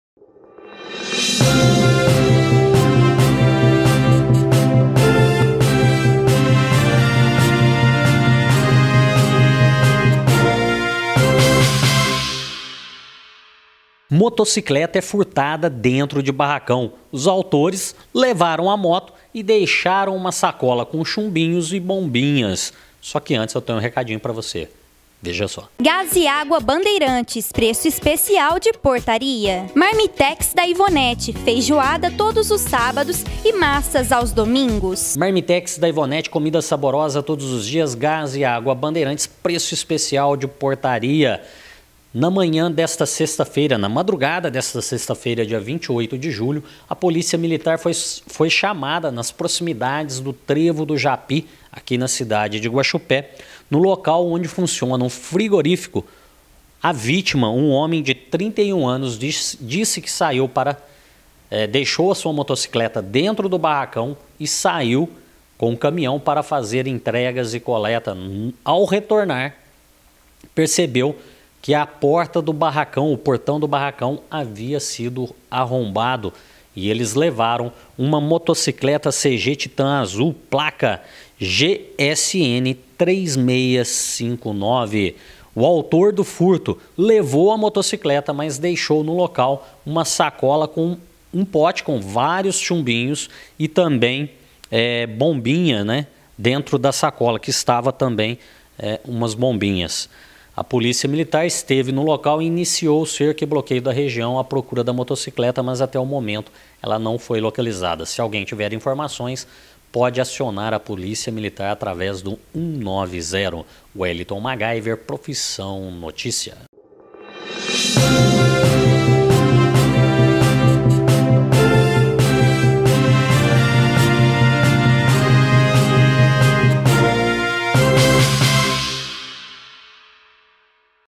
Motocicleta é furtada de dentro de barracão trancado
Autores deixaram sacola com chumbinhos e bombinhas
Na madrugada desta sexta-feira 28 de julho, a Polícia Militar foi chamada nas proximidades do trevo do Japy, na cidade de Guaxupé (MG), onde funciona um frigorífico. No local a vítima de 31 anos, disse que estacionou sua moto no barracão da empresa e o mesmo foi trancado e ao retornar de viagem, percebeu que haviam arrombado o barracão e levaram sua motocicleta, Titan, azul, placa GSN-3659. O autor do furto levou a motocicleta e deixou uma sacola com chumbinhos utilizados em espingarda, espoletas e bombinha, sendo tudo apreendido. A Polícia Militar iniciou rastreamento aos suspeitos que não foram localizados.